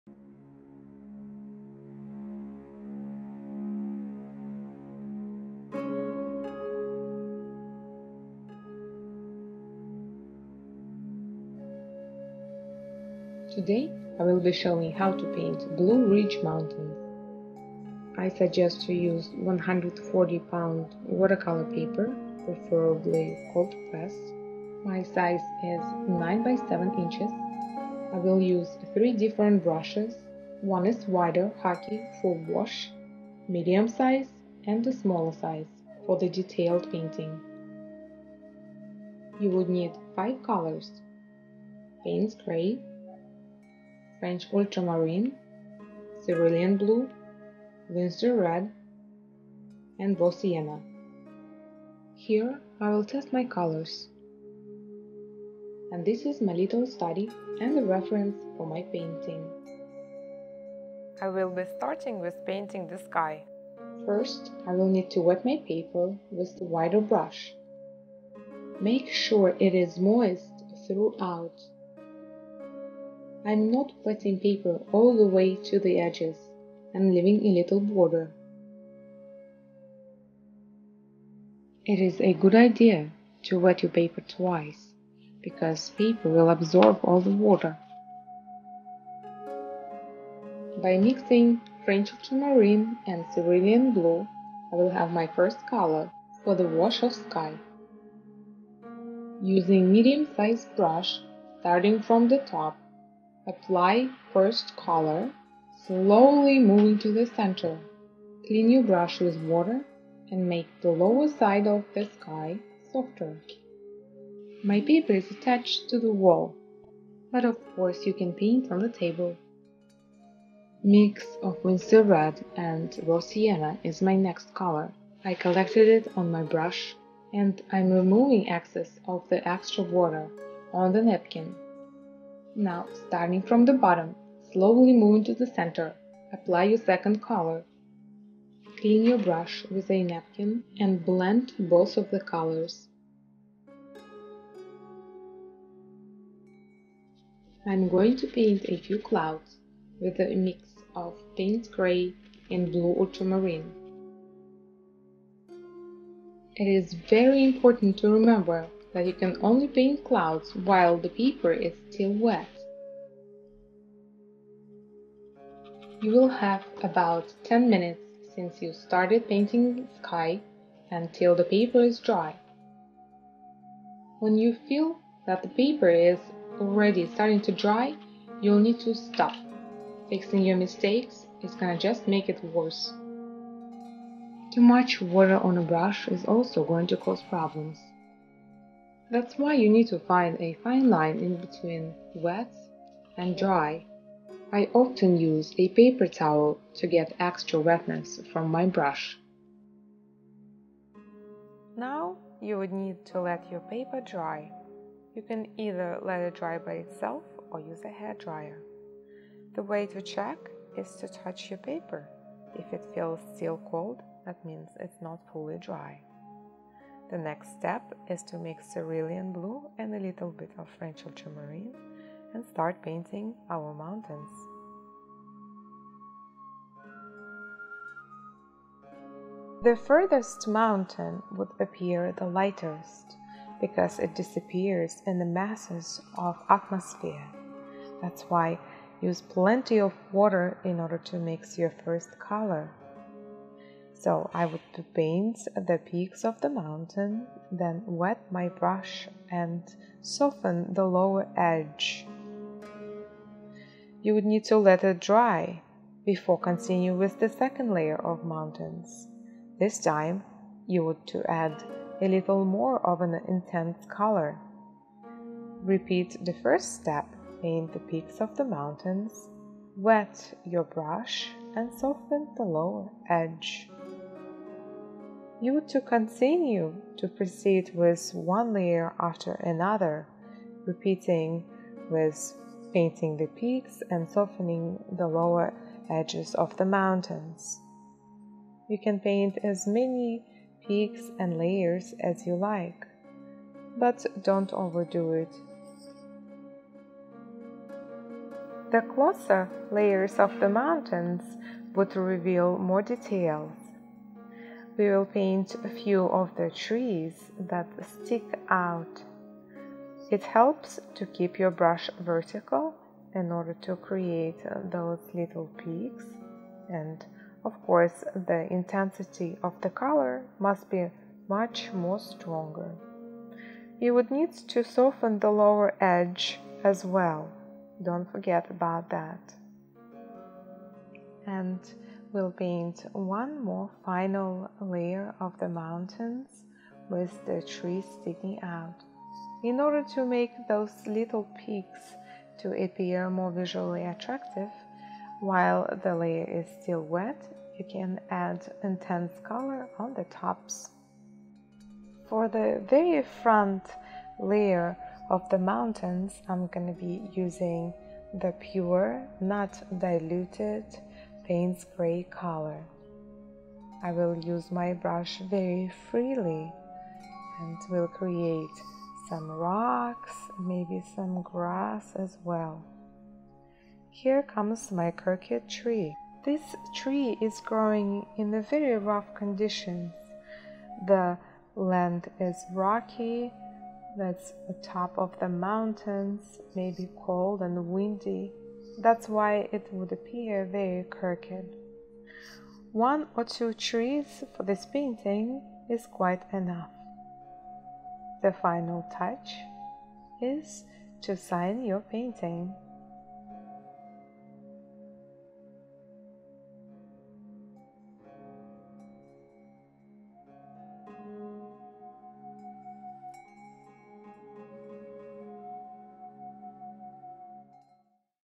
Blue Ridge Mountains. Easy step by step watercolor tutorial (updated)
In this 7 minute tutorial you will study how to paint sky and mountains using basic watercolor techniques.
Step by step instructions are very easy to follow!

Materials:
140 lb watercolor paper,
5 watercolors (Payne's Grey, French Ultramarine, Cerulean Blue, Winsor Red, Raw Sienna),
paper towel
Brushes:
  - small and pointy (N7 Loew-Cornell, round 7000)
  - wider flat and pointy (1" Silver Black Velvet, 3009S Oval)